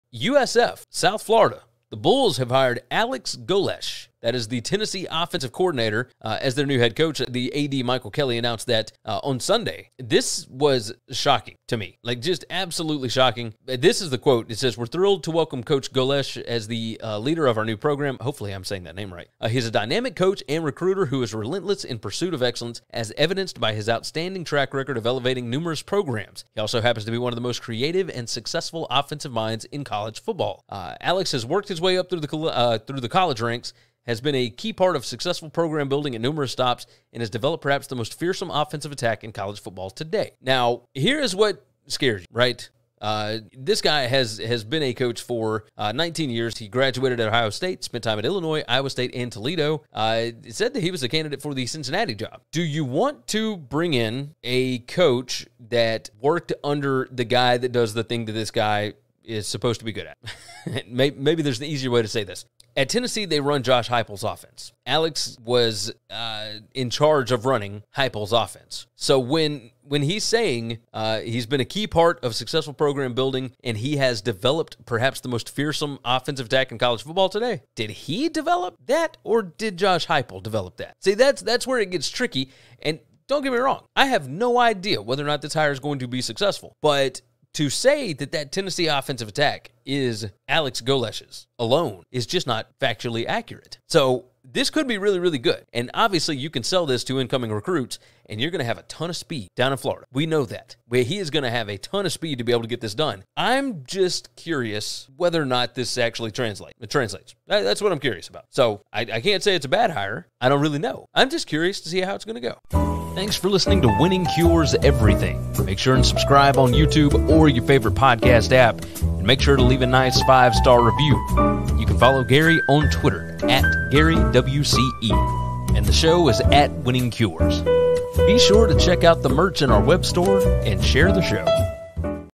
What should USF expect after hiring Alex Golesh as new football coach?
What are the expectations for USF football after they hired Tennessee OC Alex Golesh as new head coach? I don't know that it's always a good idea to hire the assistant coach that coaches the side of the ball that his head coach coaches. if that makes sense.